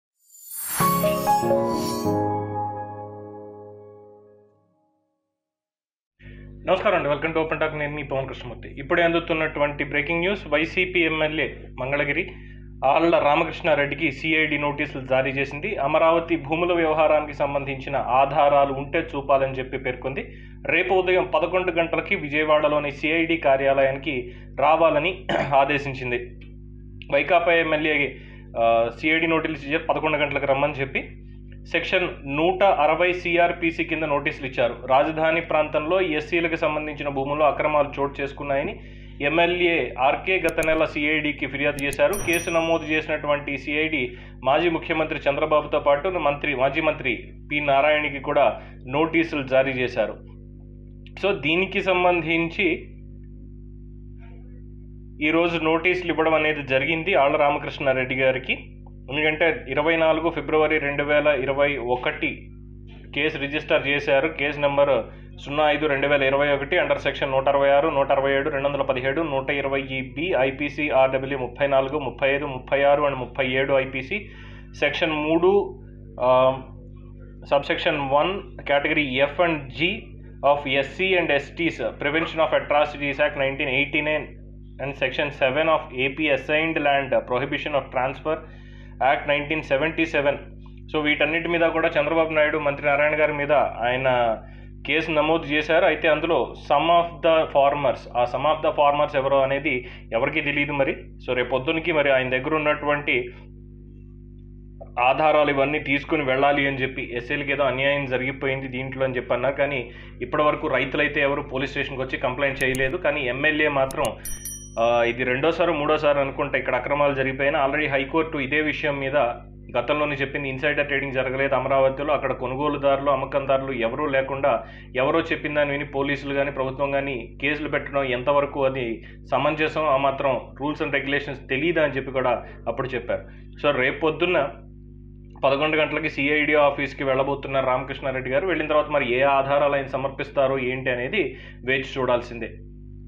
BreakingNews:ఆళ్ల రామకృష్ణా రెడ్డికి సీఐడి నోటీసులు చంద్రబాబు పై కేసులో ఆధారాలు తీసుకురమ్మని ఆదేశాలు
Breaking News : ఆళ్ల రామకృష్ణా రెడ్డికి సీఐడి నోటీసులు చంద్రబాబు పై కేసులో ఆధారాలు తీసుకురమ్మని ఆదేశాలు| OPEN TALK

Category
News & Politics